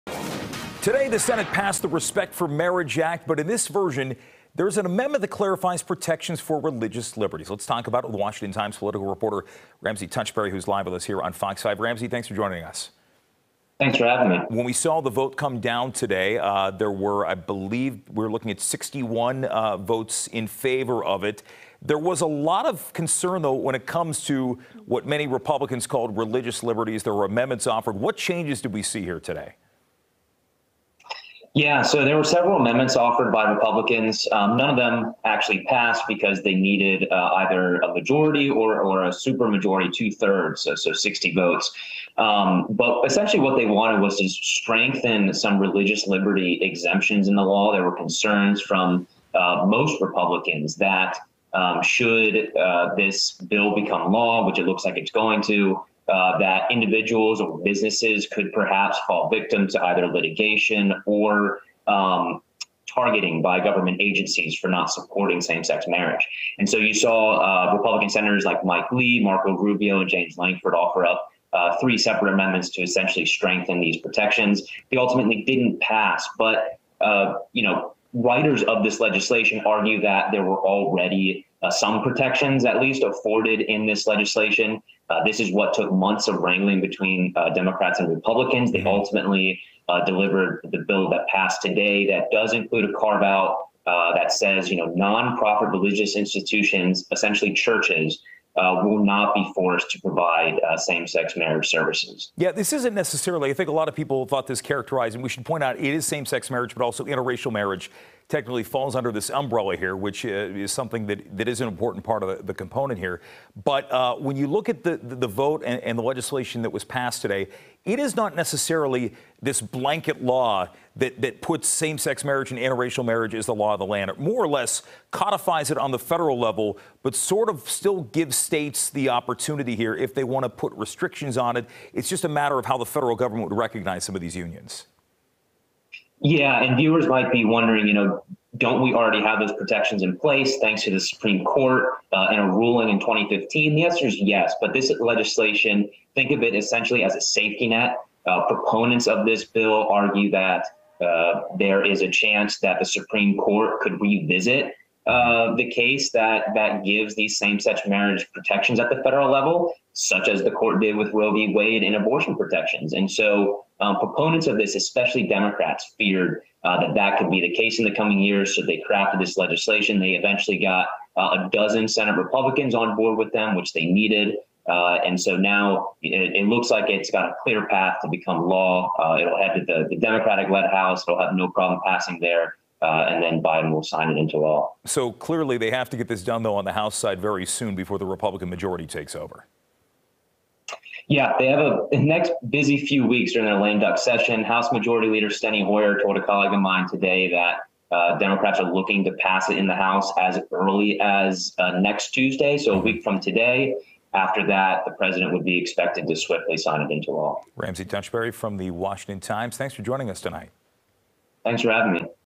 Senate passes Respect for Marriage Act | FOX 5 DC
Washington Times reporter Ramsey Touchberry joins FOX 5 to discuss the Respect for Marriage Act that the Senate passed Tuesday. If this bill becomes, law Touchberry says Republicans have their reservations.